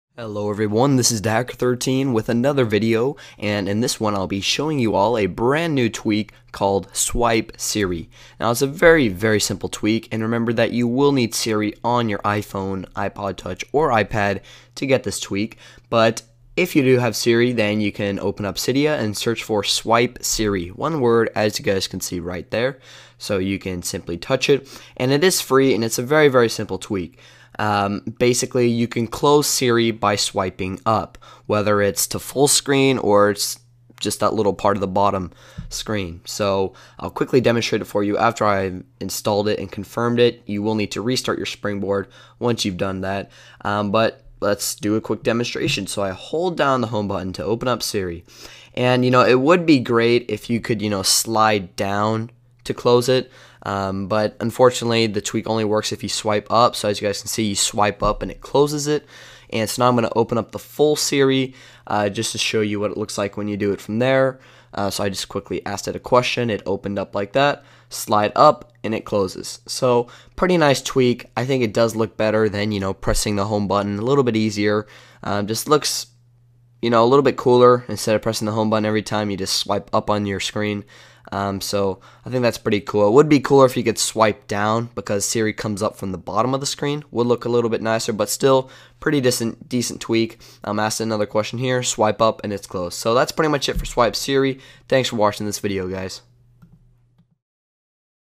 SwipeSiri | Close Siri By Swiping Up
In this quick video, I show you all a brand new tweak called 'SwipeSiri'.  This tweak allows you to exit Siri on your iPhone, iPod Touch, or iPad by simply "swiping upward" instead of pressing the Home Button.  'SwipeSiri' is currently free and can be downloaded from the BigBoss repository.